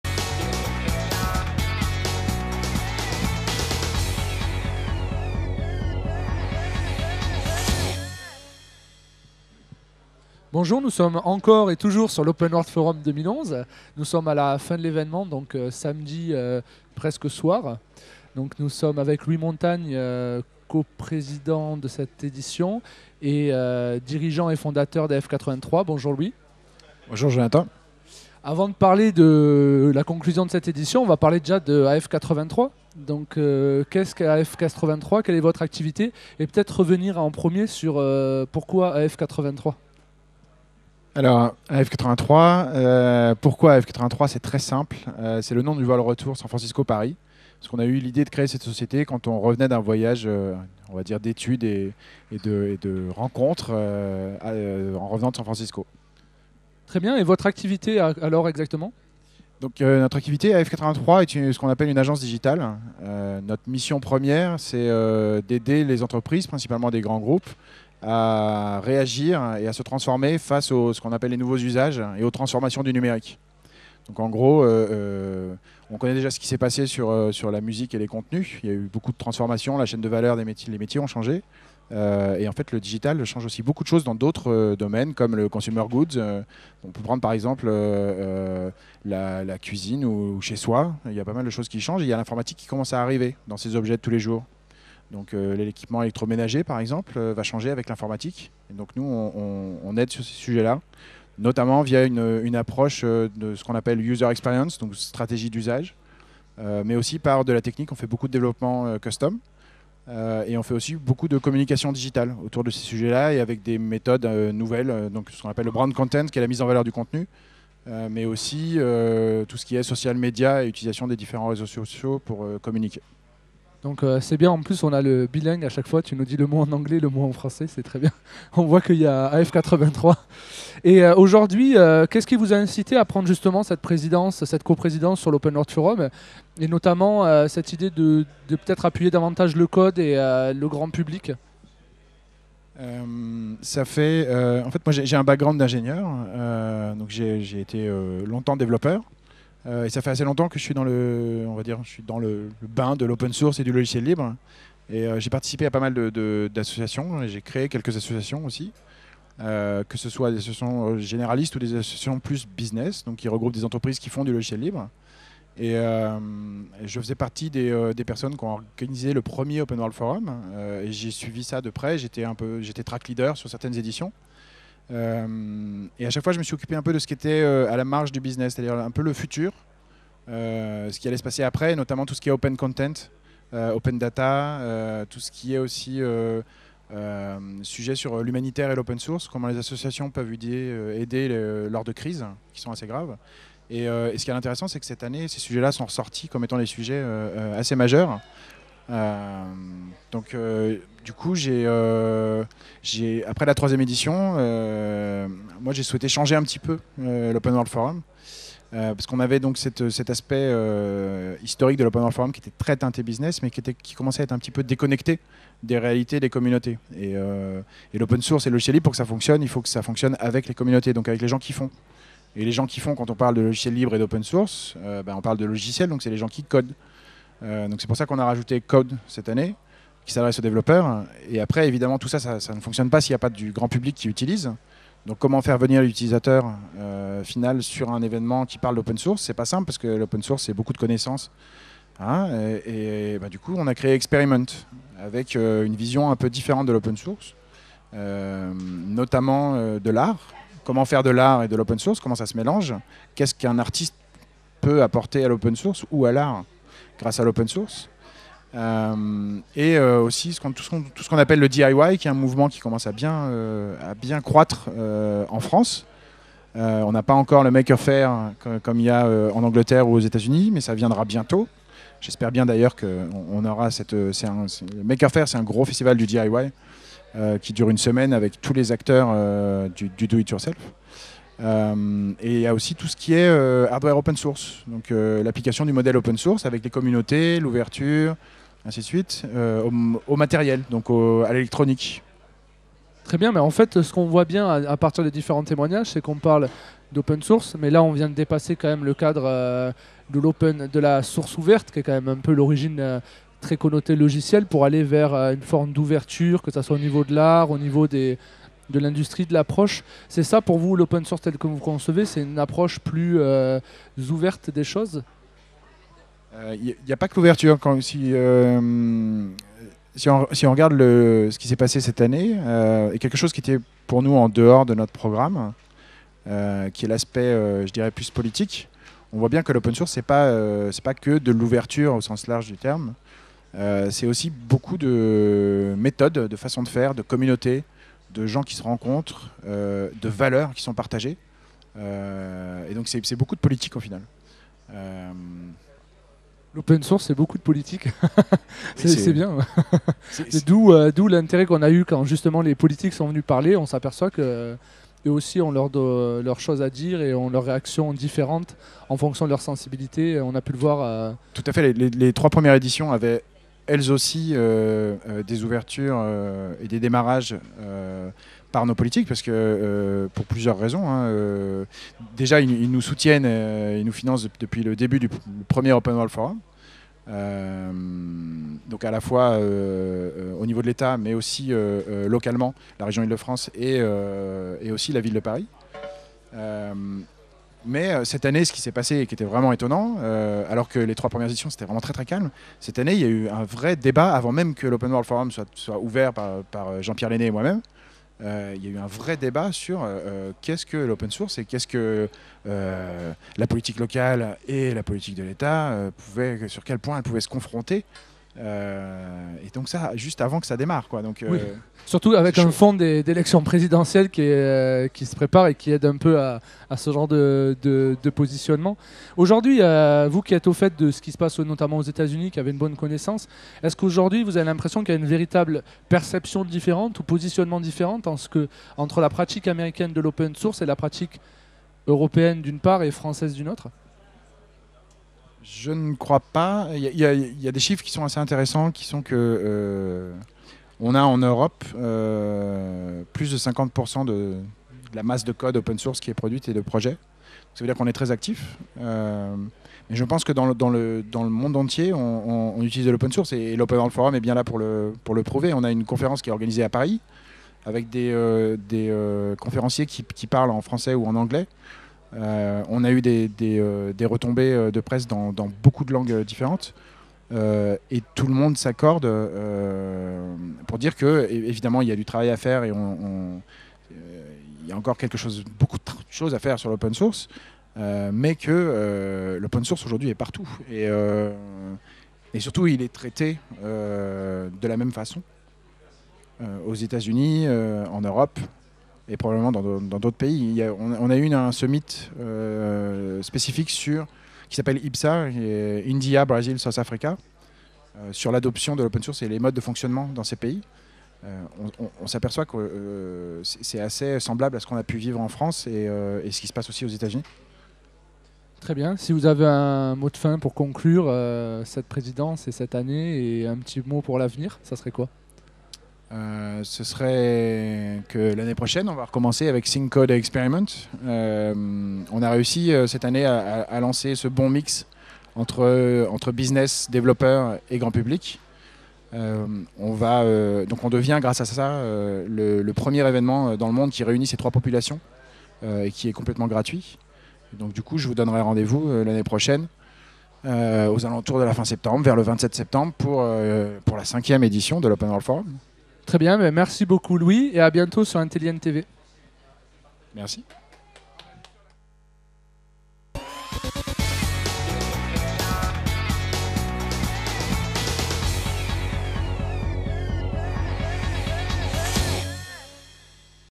Bilan Open World Forum 2011 par Louis Montagne, co-fondateur de AF83 et co-président de l'OWF 2011
Bilan Open World Forum 2011 par  Louis Montagne, co-fondateur de AF83 et co-président de l'OWF 2011. Il est interviewé par Jonathan Le Lous, Intelli'N TV